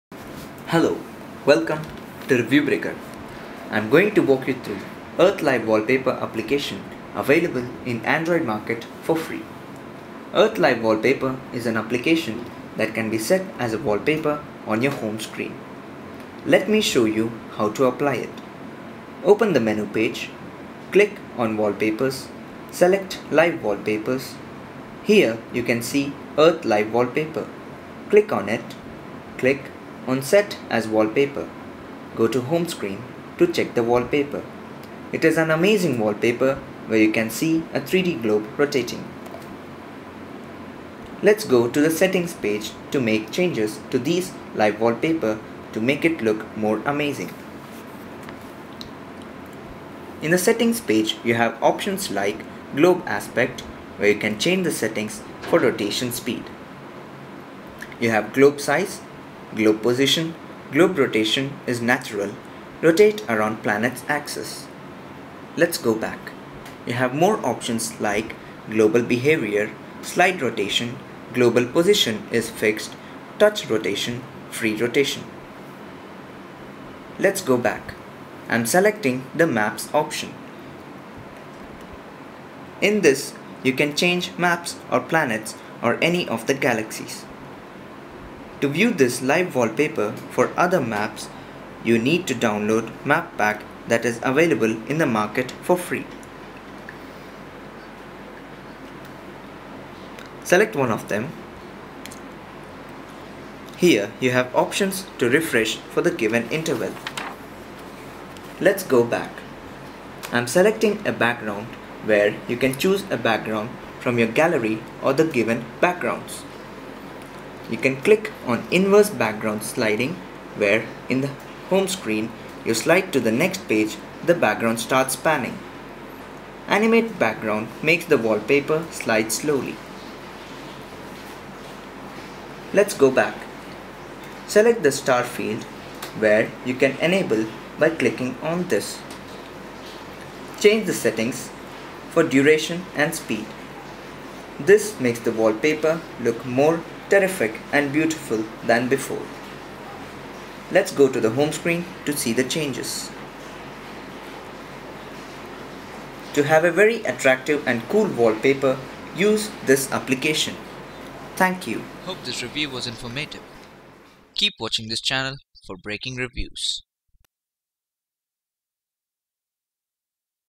Earth live wallpaper - Android App Review By ReviewBreaker
Show planets on your screen
see the site for full desc.
!! YOUR PHONE MUST SUPPORT LIVE WALLPAPERS !!
!! LAUNCH THEM LIKE WALLPAPERS, NOT APPLICATIONS !!
!! IT'S BETTER TO INSTALL THE MAP PACK !!